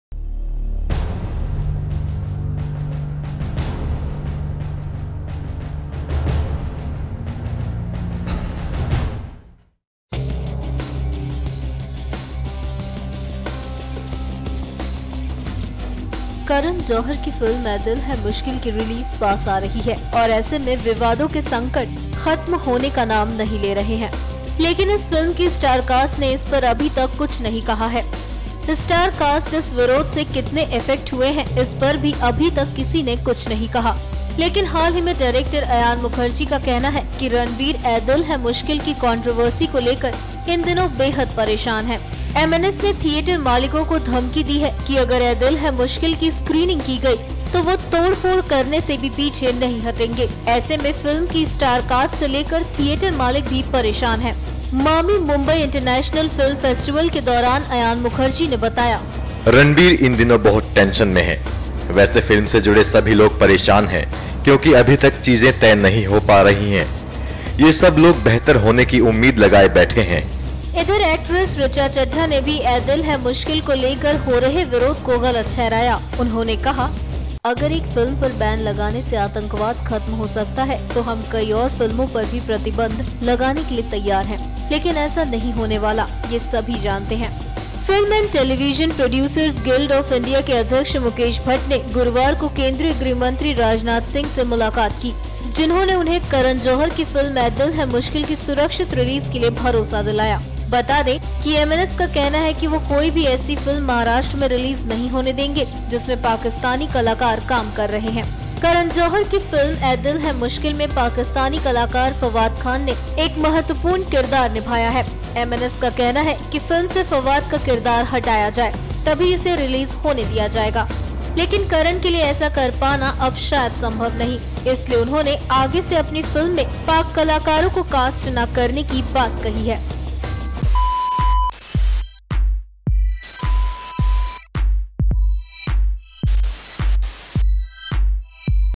Ayan Mukerji ने खोला राज़ इस वजह से है Ranbir Kapoor बड़े परेशान
Director Ayan Mukerji has said that his close friend, actor Ranbir Kapoor is worried about the controversy surrounding the release of his upcoming romantic drama 'Ae Dil Hai Mushkil'.Owing to its casting of Pakistani actor Fawad Khan, the movie has drawn ire of Maharashtra Navnirman Sena which has threatened to stall its release in theatres, slated to release during Diwali on October 28."Ranbir is tensed, everyone is worried about how things are unfolding. He is hoping for the best," Ayan told reporters here at the opening of 18th Jio MAMI Mumbai International Film Festival.